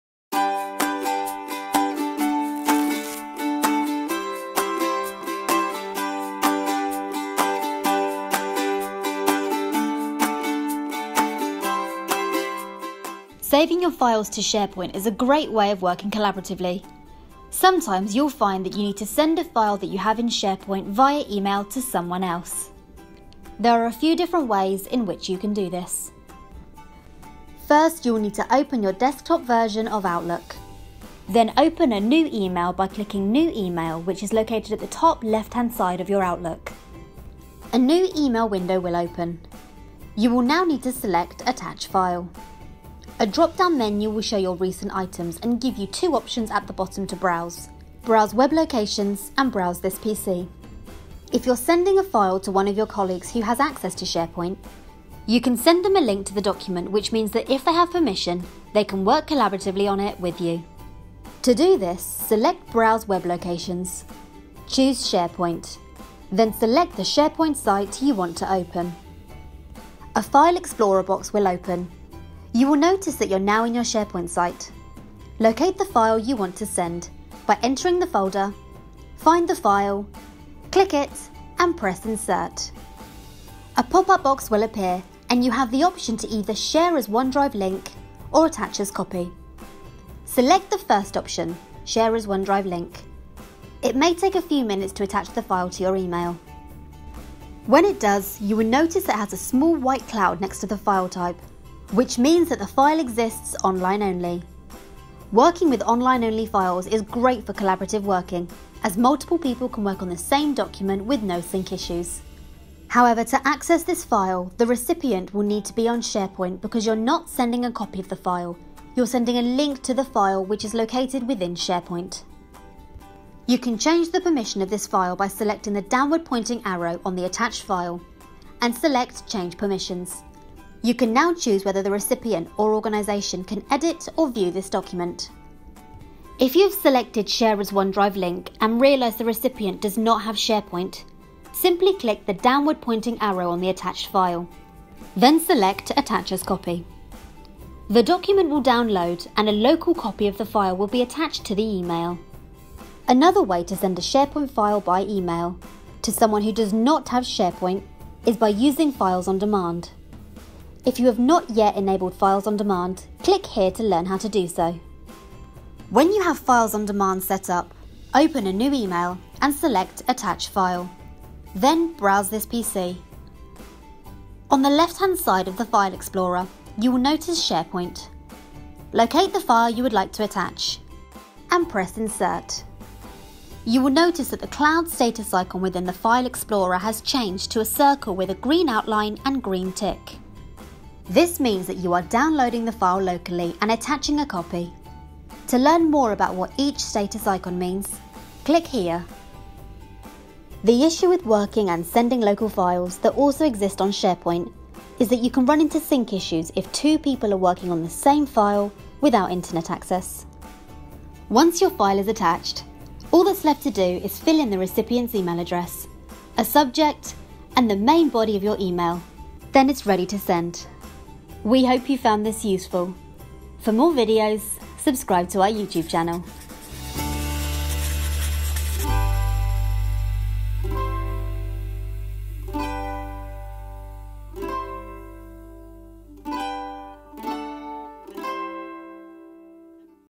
Sending a SharePoint file by email
SharePoint is a great tool within Office 365 which allows you to work collaboratively within your organisation.

In this video we will show you how to attach a file that exists on SharePoint as a link to an email that you are sending internally to someone who has access to SharePoint.

We will also touch upon how to attach a SharePoint file to an email that you need to send to someone who does not have internet access or does not have SharePoint.

We also speak, briefly about Files on Demand. If you need to enable Files on Demand take a look at our training video

If you want to know more about the File on Demands status icons, not a problem...